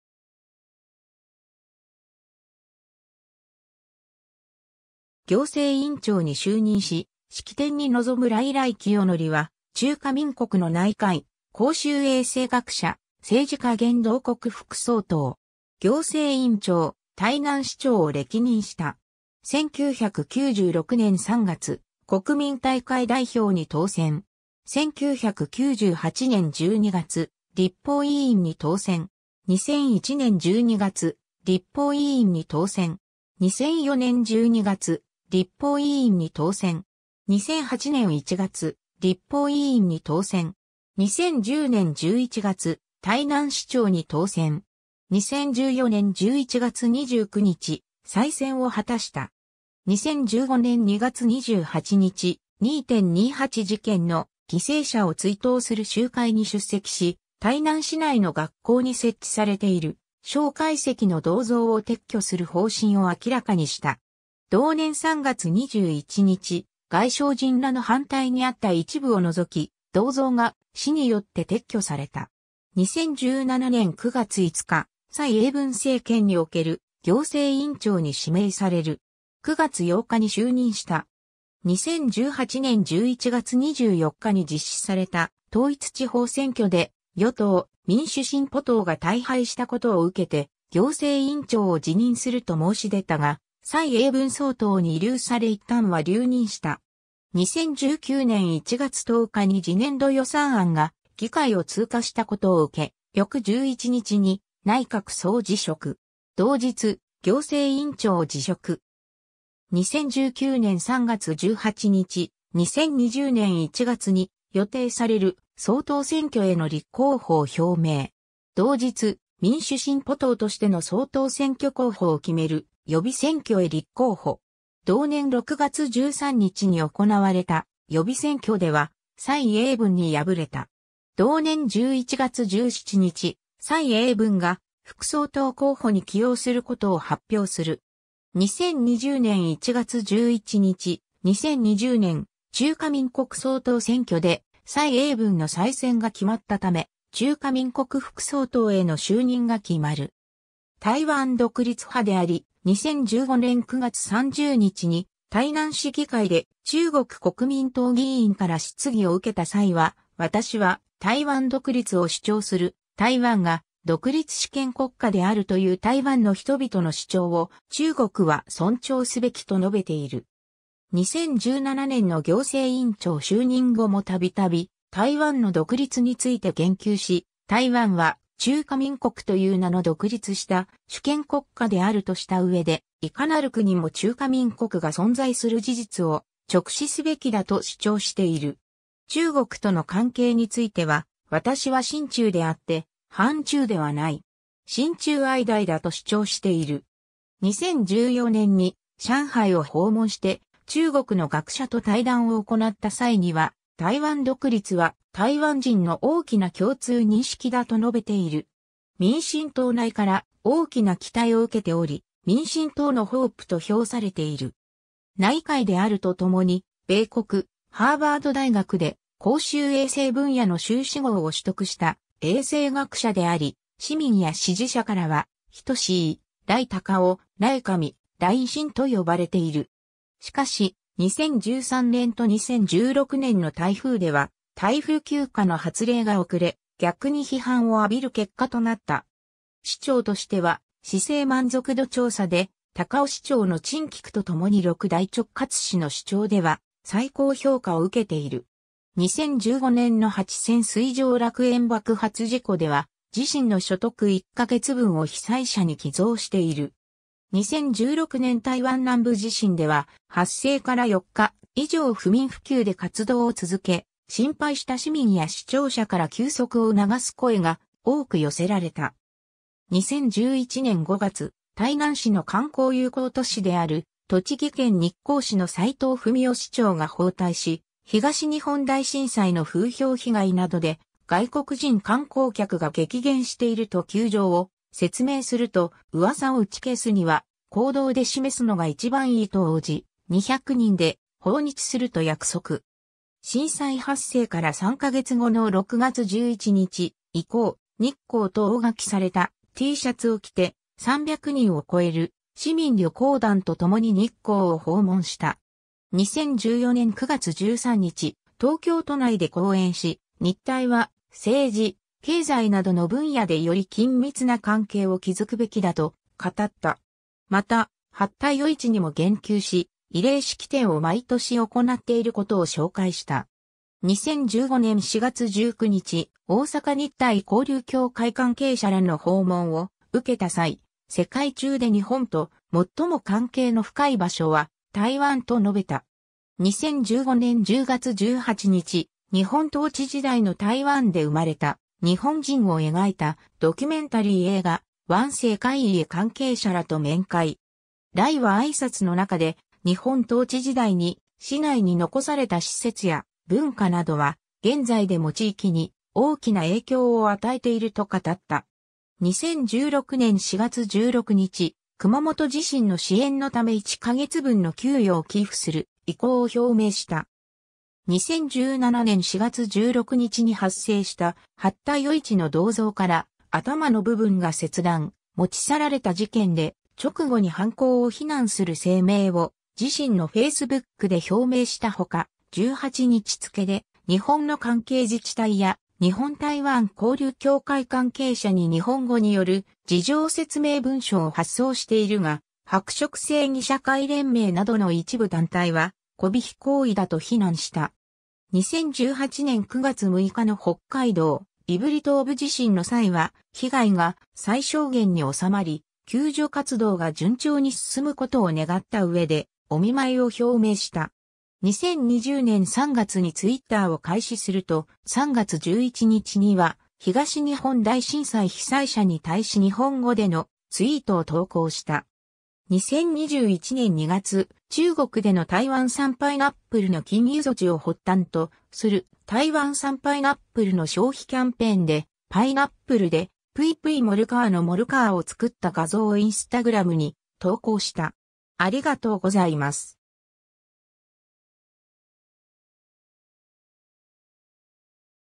頼清徳
行政院長（首相）に就任し、式典に臨む頼 頼 清徳（らい せいとく、William Lai、1959年10月6日 - ）は、中華民国の内科医、公衆衛生学者、政治家（民主進歩党）現同国副総統。
行政院長（首相に相当）、台南市長（2期）を歴任した。
 1996年3月、国民大会代表（第3期）に当選。
 1998年12月、立法委員（第4期）に当選。
 2001年12月、立法委員（第5期）に当選。
 2004年12月、立法委員（第6期）に当選。
 2008年1月、立法委員（第7期）に当選。
 2010年11月、台南市長に当選。
 2014年11月29日、再選を果たした。
 2015年2月28日、二・二八事件の犠牲者を追悼する集会に出席し、台南市内の学校に設置されている蔣介石の銅像を撤去する方針を明らかにした。
同年3月21日、外省人らの反対に遭った一部を除き、銅像が市によって撤去された。
 2017年9月5日、蔡英文政権における行政院長に指名される。
9月8日に就任した。
2018年11月24日に実施された統一地方選挙で与党・民主進歩党が大敗したことを受けて行政院長を辞任すると申し出たが、蔡英文総統に慰留されいったんは留任した。
2019年1月10日に次年度予算案が議会を通過したことを受け、翌11日に内閣総辞職。
同日、行政院長を辞職。
 2019年3月18日、2020年1月に予定される総統選挙への立候補を表明。
同日、民主進歩党としての総統選挙候補を決める予備選挙へ立候補。
同年6月13日に行われた予備選挙では蔡英文に敗れた。
同年11月17日、蔡英文が、副総統候補に起用することを発表する。
 2020年1月11日、2020年中華民国総統選挙で、蔡英文の再選が決まったため、中華民国副総統への就任が決まる。
 台湾独立派であり、2015年9月30日に台南市議会で中国国民党議員から質疑を受けた際は「私は台湾独立を主張する」、「台湾が独立主権国家であるという台湾の人々の主張を中国は尊重すべき」と述べている。
2017年の行政院長就任後も度々台湾の独立について言及し、「台湾は中華民国という名の独立した主権国家である」とした上で、「いかなる国も中華民国が存在する事実を直視すべきだ」と主張している。
 中国との関係については「私は親中であって反中ではない。親中愛台だ」と主張している。
2014年に上海を訪問して中国の学者と対談を行った際には「台湾独立は台湾人の大きな共通認識だ」と述べている。
 民進党内から大きな期待を受けており、「民進党のホープ」と評されている。
 内科医であるとともに、米国・ハーバード大学で公衆衛生分野の修士号を取得した衛生学者であり、市民（主にインターネット上）や支持者からは「仁医」、「頼隆夫」（映画KANOの宣伝で訪台した八田與一役の大沢たかおと並んだ姿がよく似ていると言われたことや、大沢もJIN-仁-では医者役を演じていたことから）、「頼神、LINE神」（市政の情報発信ツールとしてLINEを頻繁に使用し、6大直轄市市長では最もそのフォロワー数が多いことや、他の県市が台風警戒中で公的機関の閉鎖と学校の休校を打ち出す中、ただ一人逆の宣言を行い、実際に台風による被害がほぼなかったこと、その日は未明まで帰宅しなかったこと、公務での移動に使う台湾高速鉄道で居合わせた急病人の治療措置を車内で行ったことが何度かあるなどから）と呼ばれている。
しかし、2013年と2016年の台風では台風休暇の発令が遅れ、逆に批判を浴びる結果となった。
 市長としては市政満足度調査（2016年の遠見雑誌によるもの）で高雄市長の陳菊とともに6大直轄市の首長では最高評価を受けている。
 2015年の八仙水上楽園爆発事故（新北市）では自身の所得1ヶ月分を被災者に寄贈している。
 2016年台湾南部地震では発生から4日以上不眠不休で活動を続け、心配した市民や視聴者から休息を促す声が多く寄せられた。
 2011年5月、台南市の観光友好都市である栃木県日光市の斎藤文夫市長が訪台し、東日本大震災の風評被害などで外国人観光客が激減していると窮状を説明すると、「うわさを打ち消すには、行動で示すのが一番いい」と応じ、200人で訪日すると約束。
震災発生から3ヶ月後の6月11日、「行こう日光」と大書きされたTシャツを着て、300人を超える市...